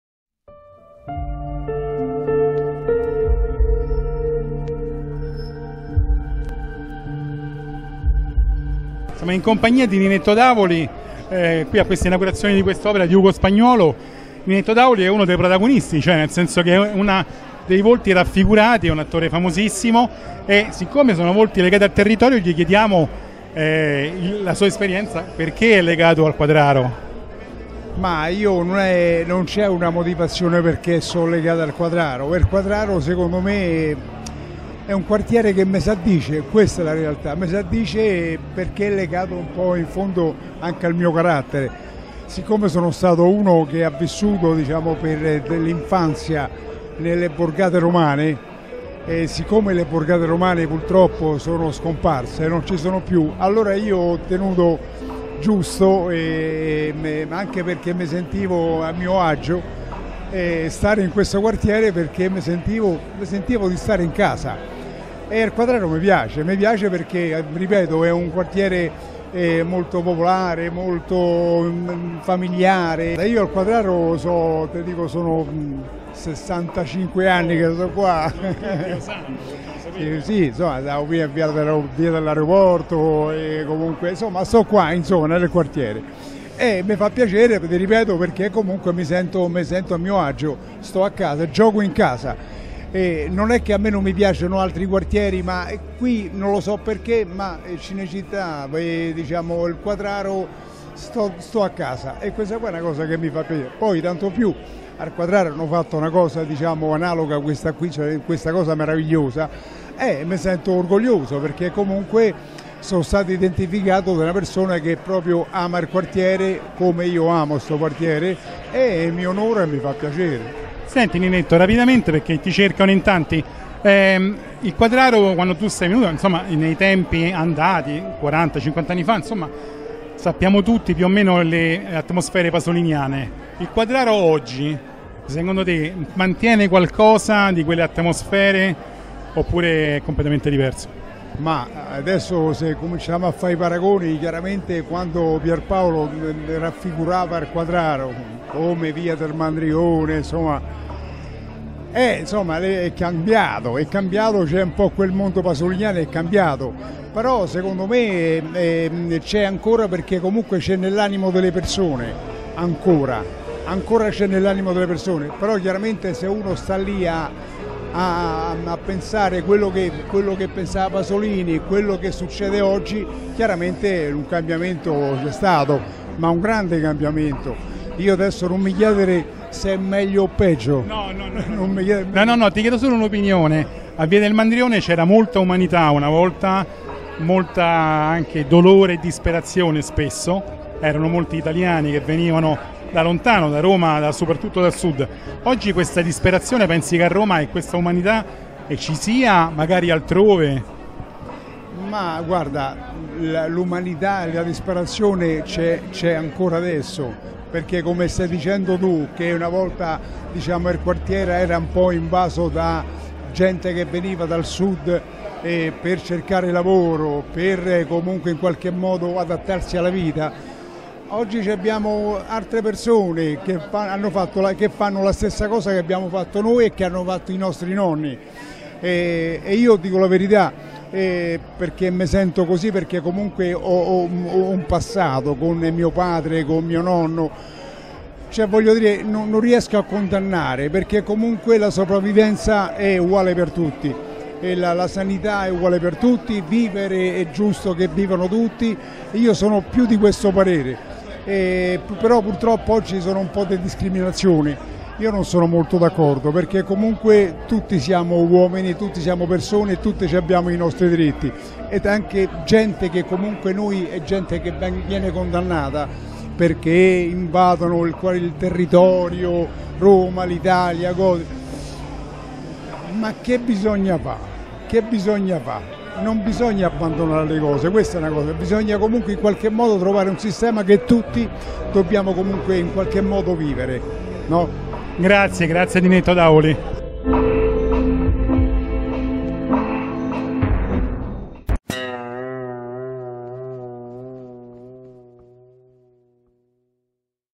Intervista a Ninetto Davoli - VUOTI DI MEMORIA di Ugo Spagnuolo
Registrazione effettuata da liberi.tv il 20 settembre 2018

Presentazione del Murale VUOTI DI MEMORIA di Ugo Spagnuolo

Intervista a Ninetto Davoli, attore residente al Quadraro e rappresentato nell'opera VUOTI DI MEMORIA di Ugo Spagnuolo.

Il Progetto artistico, voluto e patrocinato dal Municipio VII di Roma, è realizzato negli ambienti della stazione della Metro A Porta Furba - Quadraro, grazie al contributo dell’artista Ugo Spagnuolo e a cura dell’Associazione Porto d'Arte e Takeawaygallery, ed è la prima iniziativa di Arte Urbana creata con l’ausilio dei cittadini e degli abitanti del posto.
La finalità dell'operazione, che ha visto l'allestimento di un murale di oltre 400 mq, era quella di riqualificare uno spazio comune inteso come non luogo, semplice passaggio, grazie a un’opera pubblica e relazionale.
Diciotto personaggi significativi del quartiere sono stati immortalati in dimensioni monumentali nella quotidianità di un gesto altamente simbolico: la chiusura di alcune bottiglie che nell'opera diventano conserve-documenti da riempire attraverso la propria storia. Il dipinto è inoltre concepito per essere completato, nel tempo, da chi sul Quadraro insiste e vi è legato, persone che l’artista ha incontrato da febbraio 2018 effettuando laboratori in collaborazione con il Liceo Artistico G.C. Argan di Roma ed il Liceo Artistico A. Frammartino di Monterotondo e nelle sedi più varie.
Centinaia di sagome di bottiglie sono colmate quindi dai loro ricordi, trasformando così una stazione della metropolitana in un palcoscenico rivelatore di memoria, privata o collettiva, storica o intima, ove recuperare immagini, reminiscenze, identità.
Il murale è finanziato grazie alla liberalità di privati e aziende che si sono attivati nel tempo. Alle sponsorizzazioni di alcune importanti realtà economiche locali, prima fra tutte l’Harley Rock Crew seguita da De Quintili Panificio & Pasticceria, Elettrica Morlacco e Carlo Carra, si è unito, per quanto concerne i colori utilizzati, Sigma Coatings, attività che da anni supporta attivamente la riqualificazione dei luoghi del quartiere.